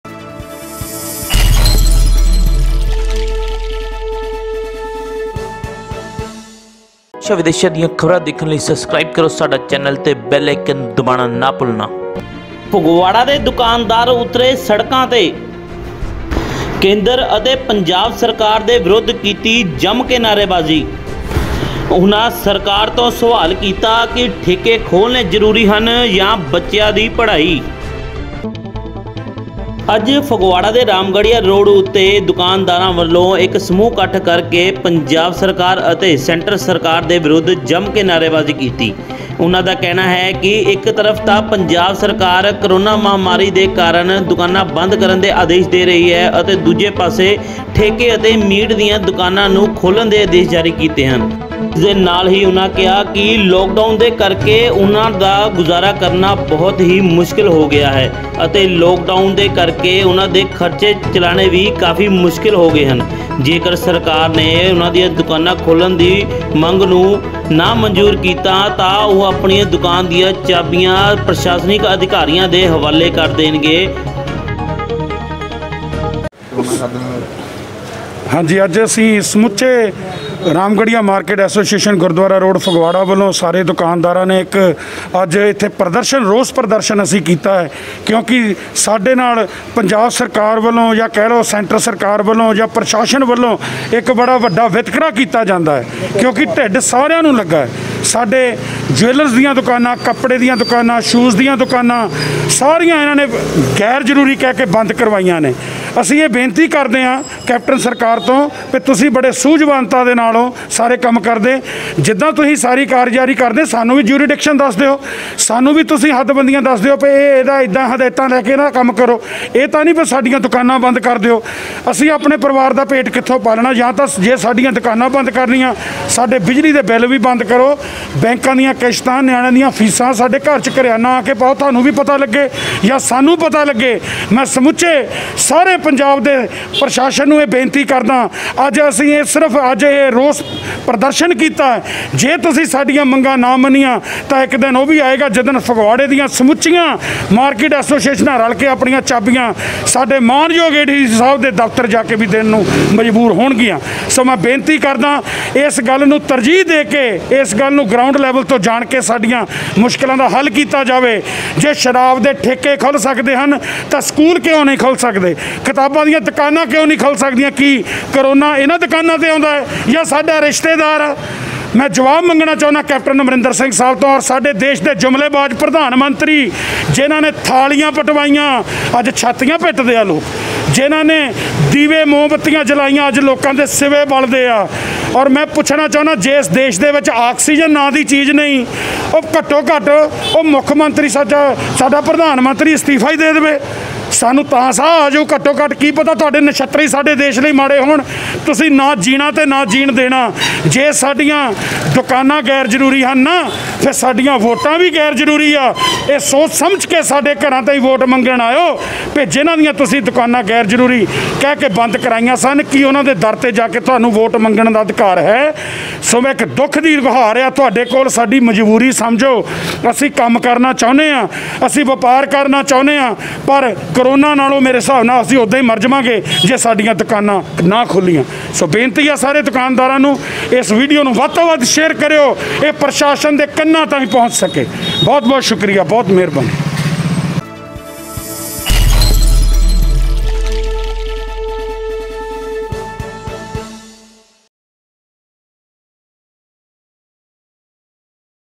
Protest || ਪ੍ਰਸ਼ਾਸਨਿਕ ਅਧਿਕਾਰੀਆਂ ਤੋਂ ਪੁੱਛਿਆ ਸਵਾਲ ਸ਼ਰਾਬ ਦੇ ਠੇਕੇ ਖੋਲਣੇ ਜਰੂਰੀ ਹੈ ਜਾਂ ਬੱਚਿਆਂ ਦੀ ਪੜਾਈ
ਫਗਵਾੜਾ ਦੇ ਦੁਕਾਨਦਾਰ ਉਤਰੇ ਸੜਕਾਂ ਤੇ, ਕਿਹਾ ਦੁਕਾਨਾਂ ਖੋਲਣ ਦੀ ਦਿੱਤੀ ਜਾਵੇ ਮੰਜੂਰੀ